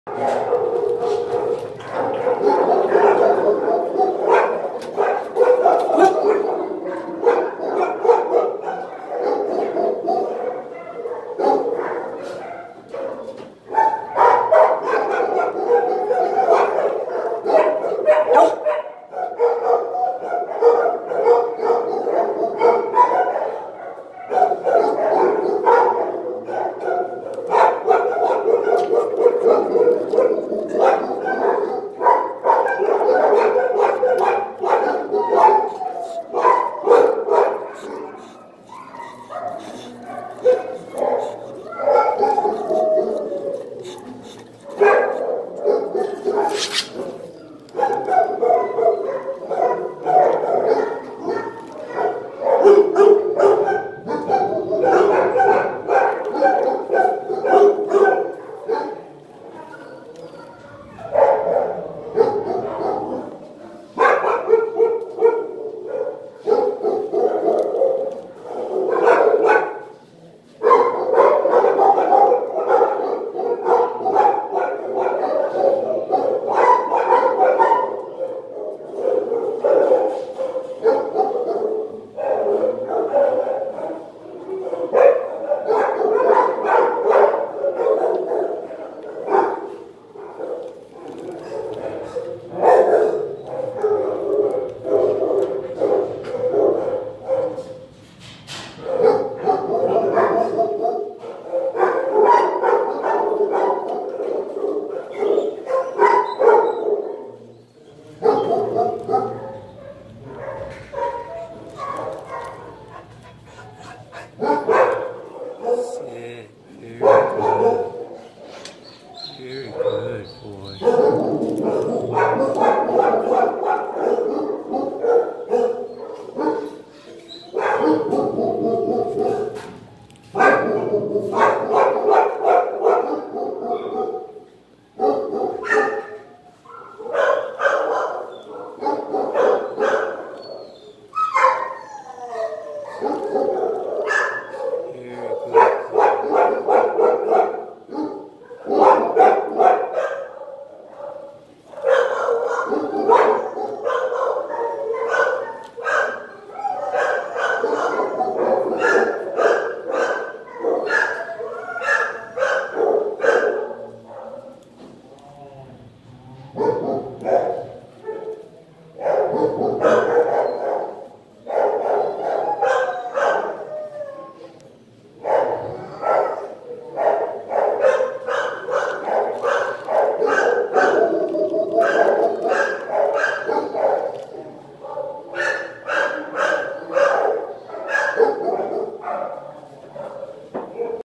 Old film❤️Upham 5 Yr Old A740424 Kennel 116 PACC shelter orange sna behavior❤️old 20210626 171601
❤️Upham 5 Yr Old A740424 Kennel 116 PACC shelter orange sna behavior❤️ Upham was a huge barker whenever I would originally come around his kennel a few weeks ago but by and by he is hopefully seeing me as reliable and a nice yummy doggie snack provider so perhaps he can avoid becoming so worked up by my presence❤️ Upham has his periods where he is fairly vocal for a good while but I believe his anxious behavior sparks because he loses our link or bond and he grows squirrelly focusing his feelings on some of his neighboring doggies ballyhoo❤️ Upham and I are facing a huge challenge because he is surrounded by some noises and flurry and we can very easily be sucked in nearby feelings❤️ Upham's focus on clamor can be pulled away if he has enough desire and since he finds my flavorful doggie munchies a good draw, he will swivel his head around and lock his regard conversely on sniffing and searching for a few savory doggie morsels I've lobbed overhead in his kennel❤️ Upham is much more open minded when he is laid back and as a consequence every now and again he will show a loving open muzzle smile, plop his rear end down for me or jump up in my face so he can snag some delicious doggie goodies I have for him in my hand❤️ Upham really is a heavenly guy, I guess he can be spurred up fairly easily and he's kind of rough around his edges but I love him and I am beginning seeing a side of him which warms my heart❤️